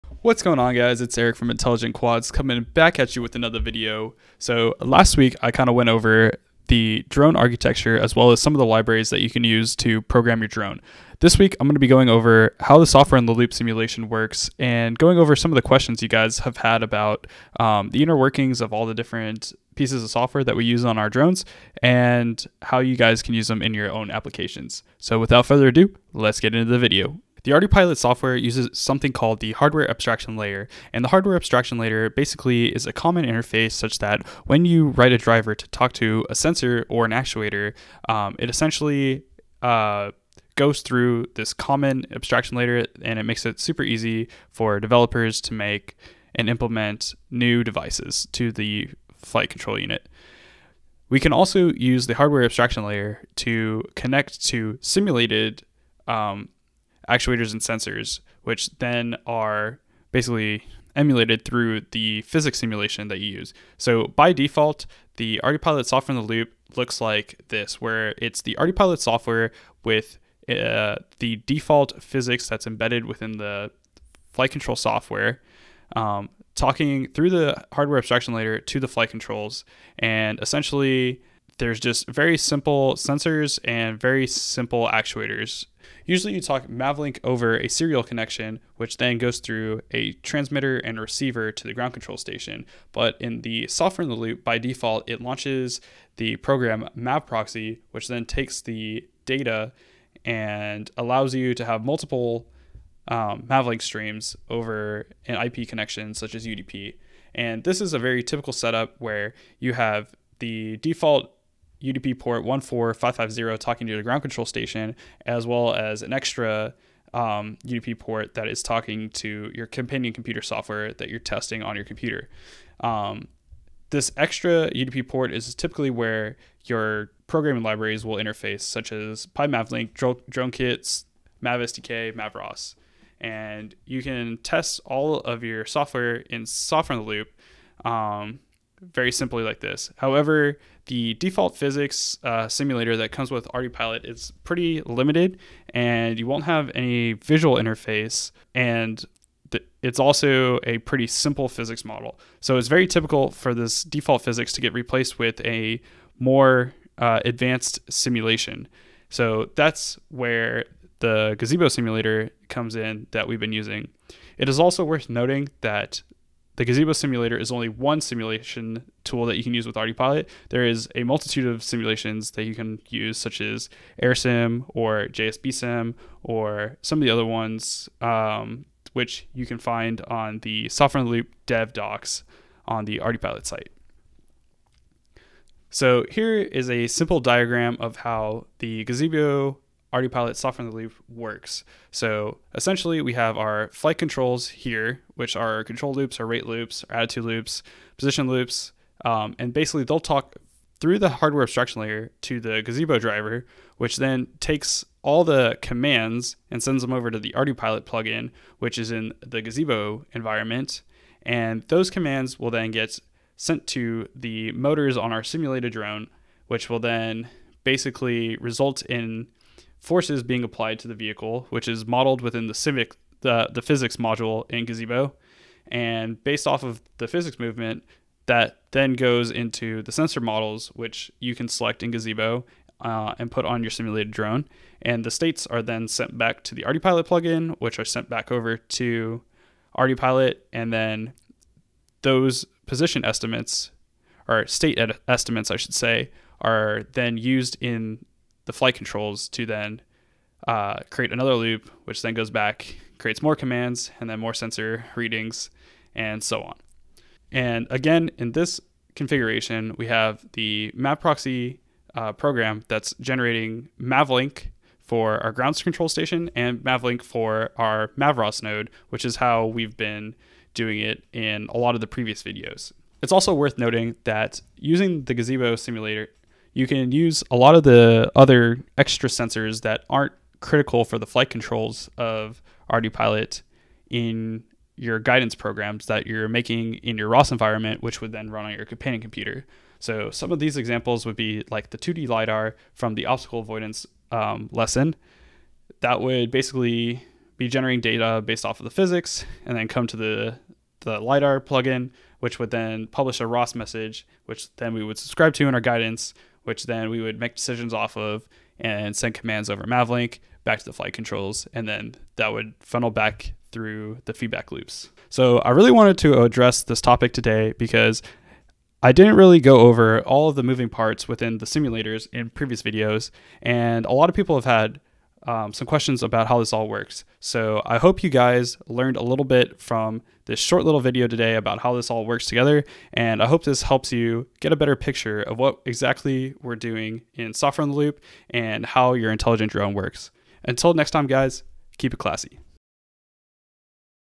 Ardupilot SITL Explanation
In this Video I show how the Ardupilot Software in the Loop (SITL) simulation works.